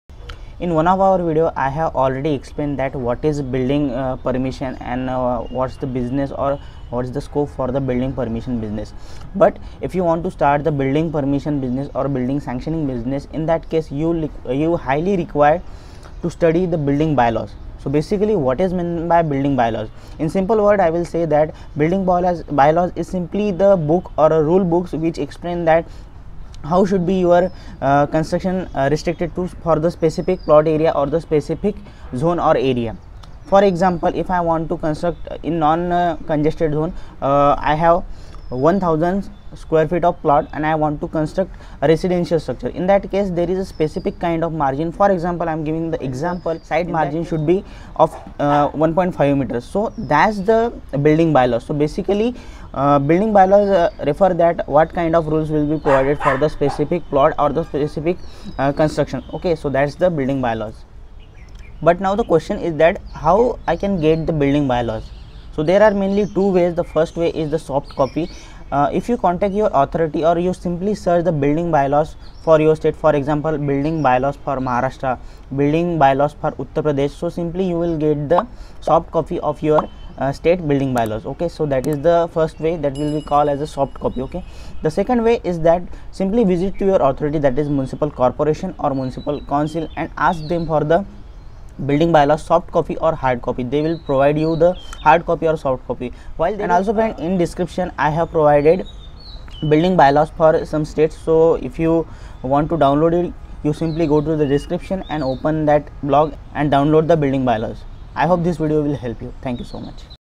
What is Building Bye Laws and How to Download Building Bye Laws by Er Suraj Laghe
Watch this video to know:

1. What is Building Bye Laws for Building Permission?

2.  How to get Building Bye Laws for Building Permission?